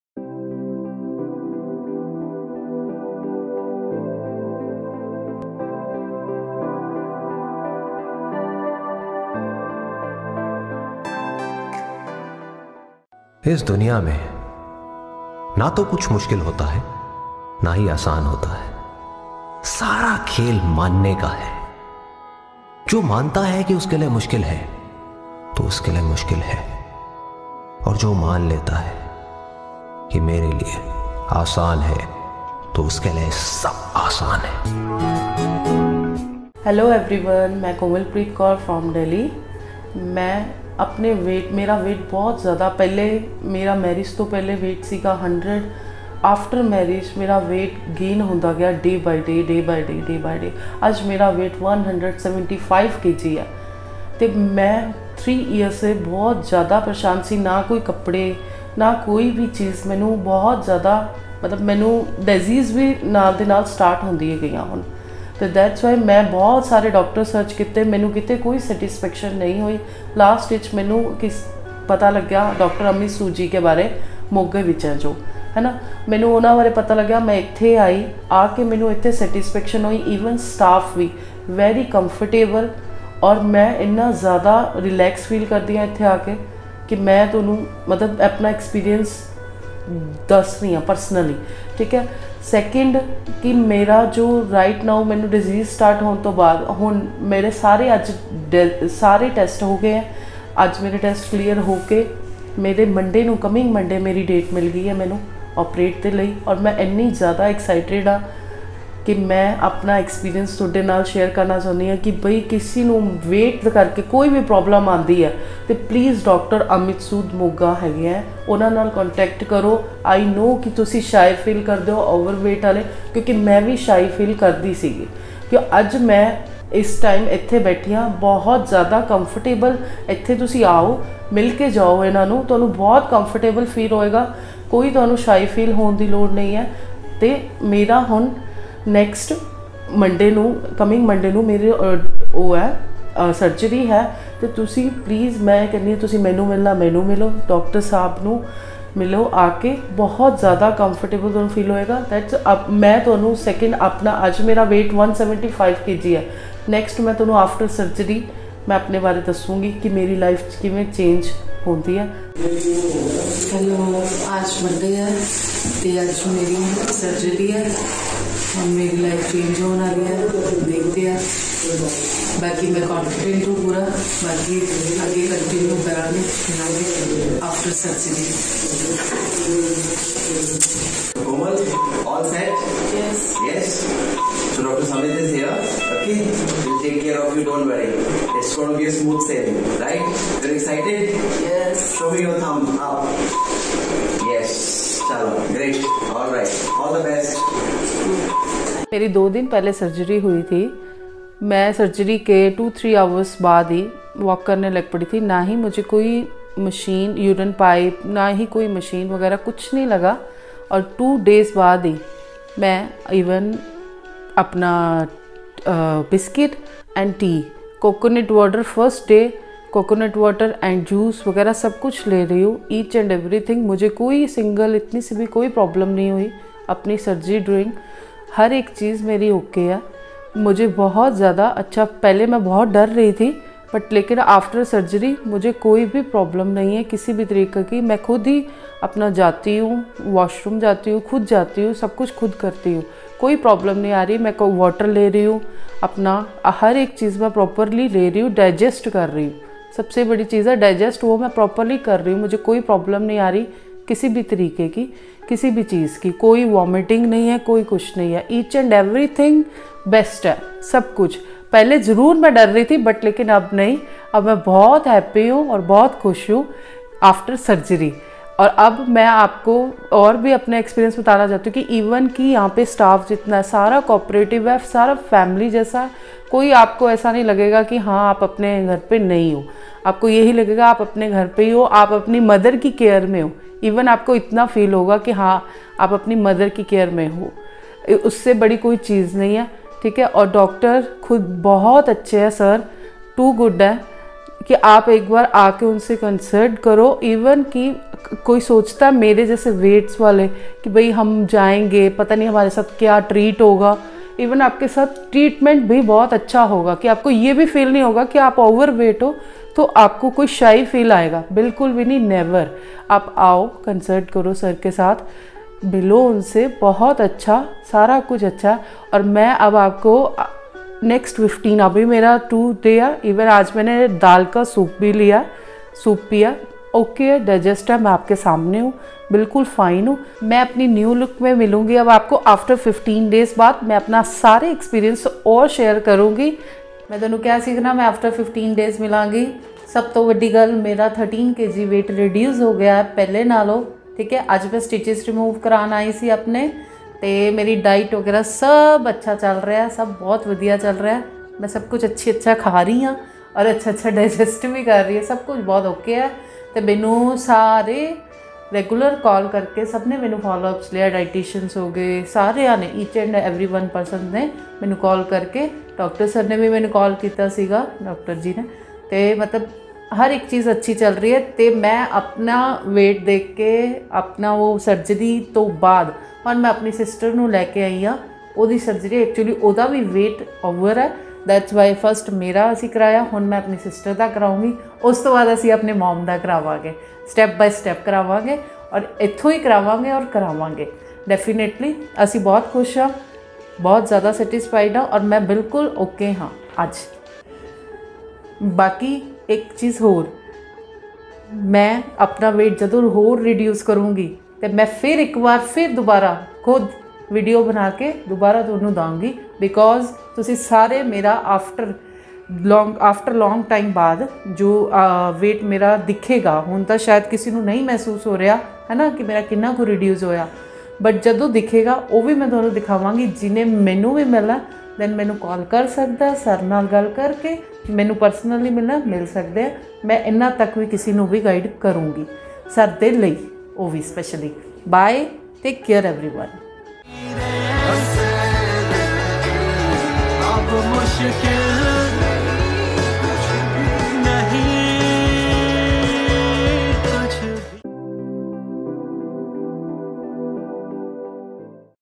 BARIATRIC WEIGHT LOSS SURGERY| DR AMIT SOOD| KOMALPREET’S STORY| BEST WEIGHT LOSS CENTER IN PUNJAB
On January 20, 2022 komalpreet underwent mini gastric bypass surgery for weight loss with Dr Amit Sood Bariatric and Metabolic surgeon at Ckosmic Surgical Centre, Moga. Komalpreet was 175 kgs when she came to consult Dr. Amit Sood. It is then she decided to undergo bariatric surgery and to gift herself a new life.

She lost 13 kgs in 15 days when she revisited the center for regular follow up.
The journey of her weight loss starts and let’s sees what she has to tell us.